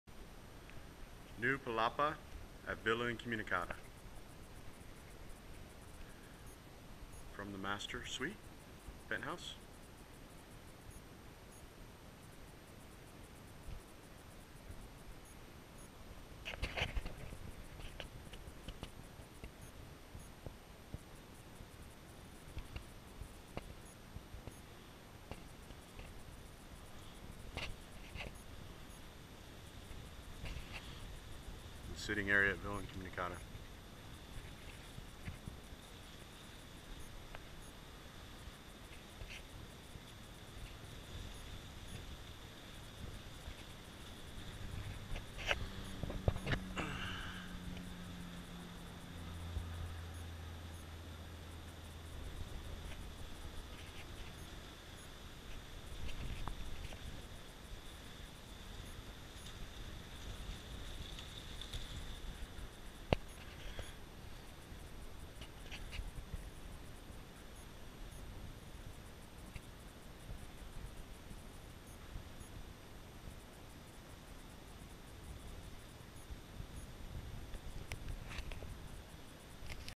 Villa Incommunicada Palapa on Roof
New Palapa on the roof of Villa Incommunicada Perfect for Wedding reception or just hanging out.